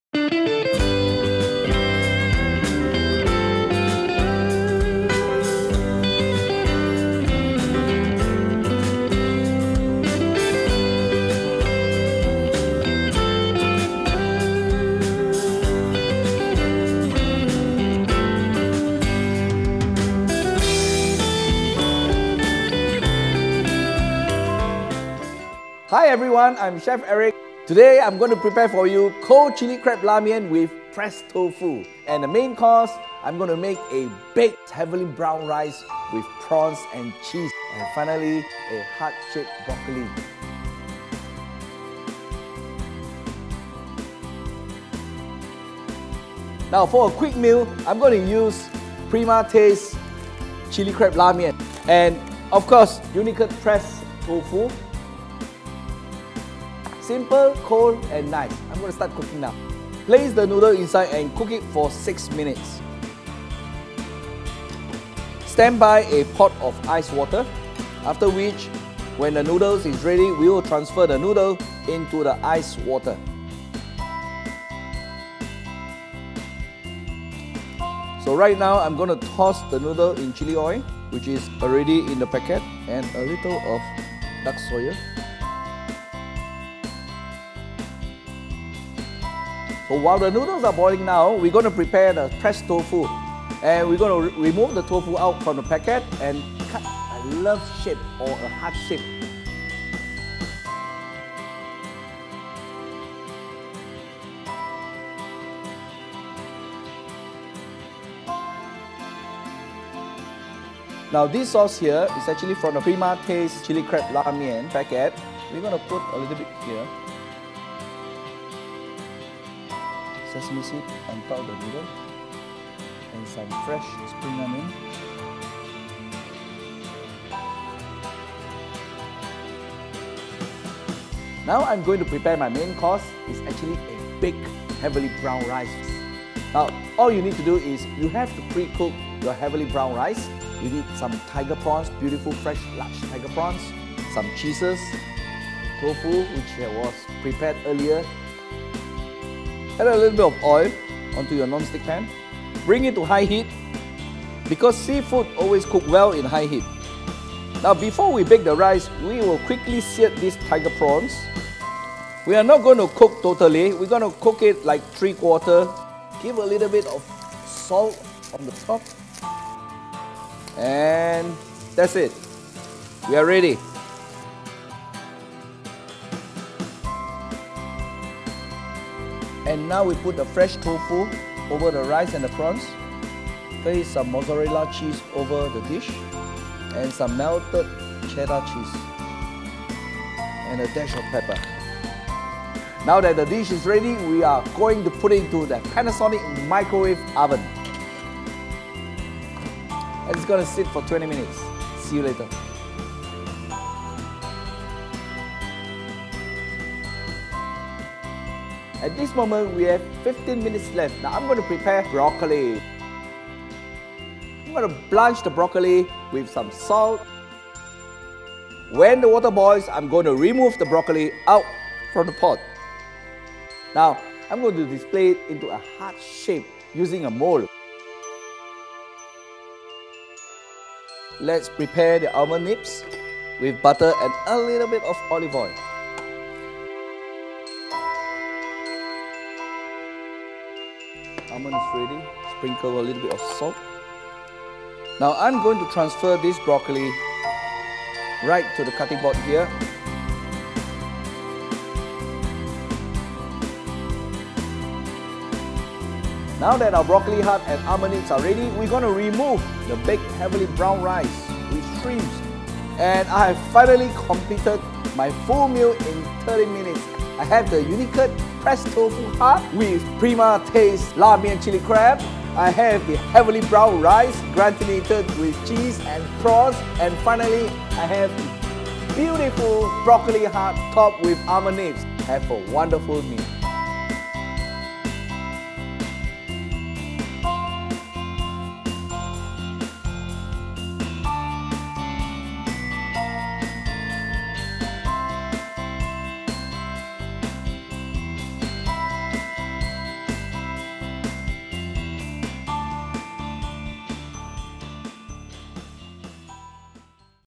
Cook a Meal in 30mins: Romantic Recipe and Tips from Celebrity Chef Eric Teo
No time to cook for your love ones, this is the recipe for you! In this video, Celebrity Chef Eric Teo is going to teach how to cook a romantic meal for your  family and love ones, and all could be achievable within 30 minutes. You don't have to be a chef or a regular cook to be able to cook these dishes. It is simple and easy.

Always pricey to eat out on Valentines' Day? How about cook up a simple yet totally delectable meal for your loved one this Valentines' Day? There's a little bit of fusion going on in the Baked Heavenly Brown Rice with Prawns and Cheese, and paired with a really easy dish like the Broccoli with Buttered Almond Nibs. Full of flavour and best thing is your partner would think it's such a complicated dish when you plate it out. Last but not least add a bit of spice into your romantic meal with the Cold Lamian Chilli Crab Sauce with Pressed Tofu. This one will totally seal the deal! ;)

Here is what you should do. Firstly, view the video to have an overview of how to cook the meal within 30 minutes. Secondly, print out the recipes for the individual dishes by clicking Thirdly, access the video for individual dishes to understand the steps before you start cooking. Have FUN guys!

Click here to bring you to the individual recipe.

0:41 - Cold Lamian Chilli Crab Sauce with Pressed Tofu

2:06 - Baked Brown Rice with Prawns and Cheese

3:23 - Broccoli with Buttered Almond Nips

So, don't wait to show your love. With 30 minutes, you could impress your love one with a little effort. To access more quick and easy meals, we have a library of 30minutecookin videos by renowned chefs.